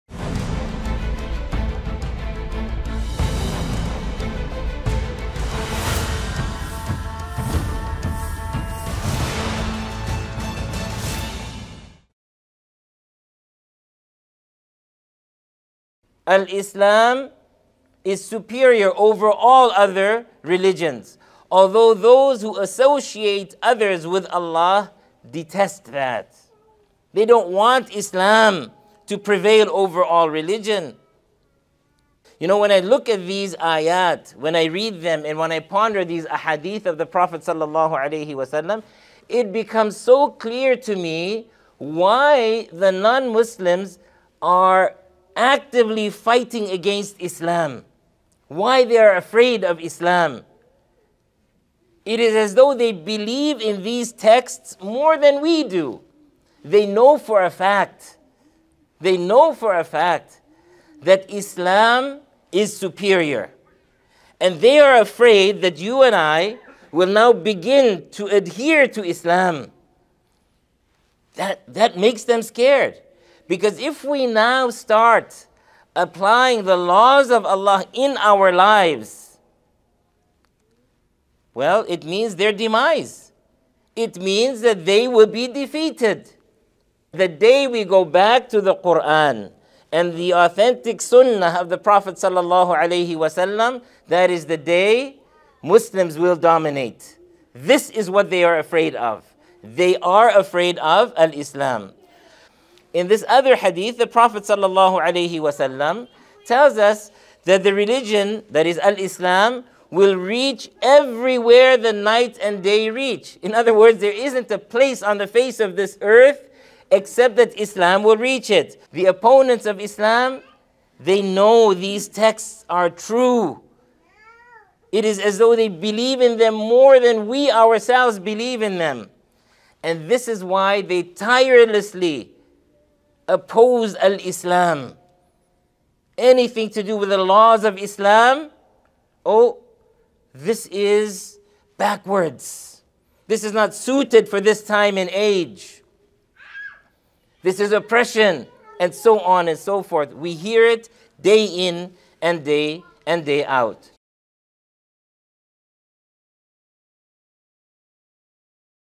Canadian Imam: Non-Muslims Know that If We Adhere to Islamic Teachings, Islam Will Rule the World
In a Friday, January 28, 2022 sermon at Muslim Youth Victoria, Canadian Imam Younus Kathrada said that non-Muslims fight against Islam and are afraid of it because they know that “Islam is superior.” He said that non-Muslims know that if Muslims adhere to Islam, this would mean their “demise” and the rise to dominance of Islam. He added that the opponents of Islam believe in the Islamic texts predicting that Islam will rule the world even more than Muslims do, and that this is the reason that they label Islam as backwards and oppressive. For more about Sheikh Younus Kathrada, see MEMRI TV Clips Nos. 9256, 9050, 8878, 8409, 8289, 8003, 7896, 7534, 7098, 6950, and 6906.